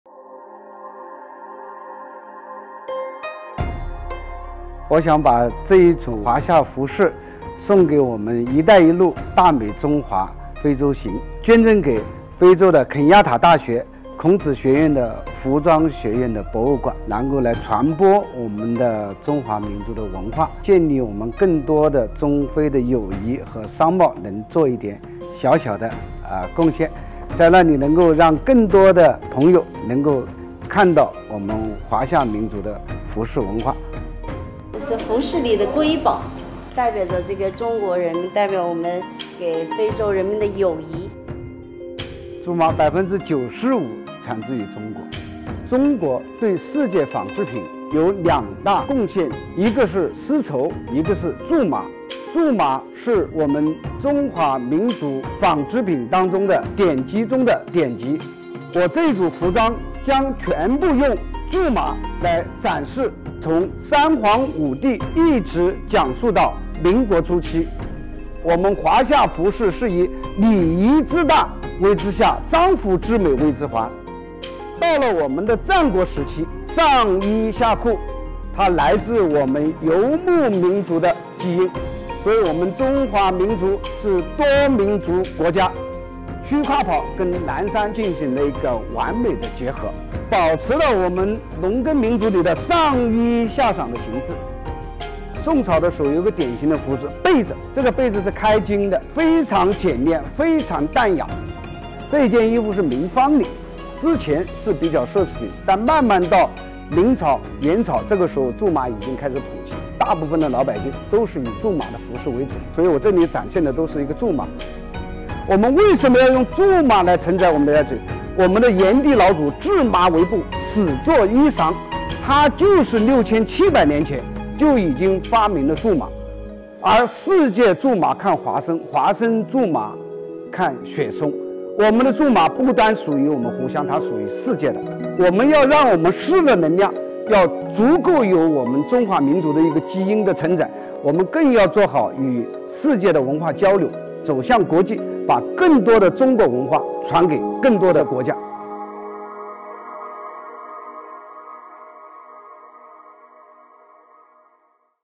公益时尚两不误！苎麻华服即将“潮”燃非洲
以公益为媒，与文化同行，携湘品“出海”，让世界看到中华服饰之美！clothing